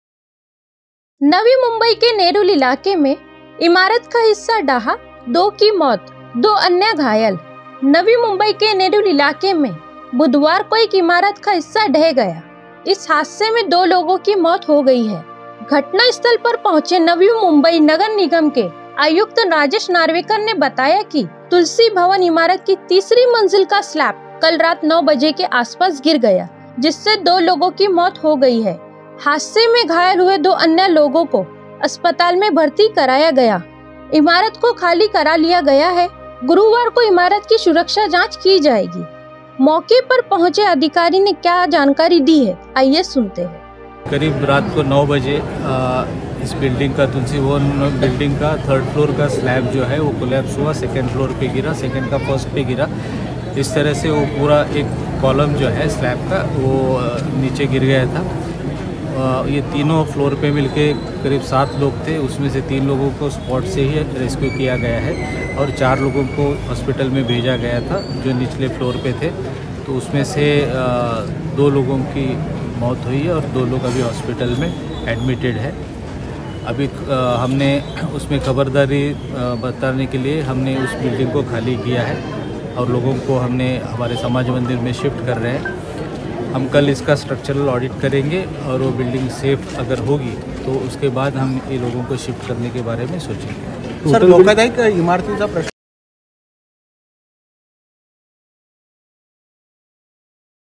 बिल्डिंग का स्लैप गिरने से दो लोगों की हुई मौत || Aapka Prahar Times
नवी मुंबई के नेरुल इलाके में बुधवार को एक इमारत का हिस्सा ढह गया। इस हादसे में दो लोगों की मौत हो गई। घटनास्थल पर पहुंचे नवी मुंबई नगर निगम के आयुक्त राजेश नार्वेकर ने बताया कि तुलसी भवन इमारत की तीसरी मंजिल का स्लैब kal रात 9 बजे के आसपास गिर गया। जिससे दो लोगों की मौत हो गई है। हादसे में घायल हुए दो अन्य लोगों को अस्पताल में भर्ती कराया गया है।
इमारत को खाली करा लिया गया है। गुरुवार को इमारत की सुरक्षा जांच की जाएगी Aapka Prahar Times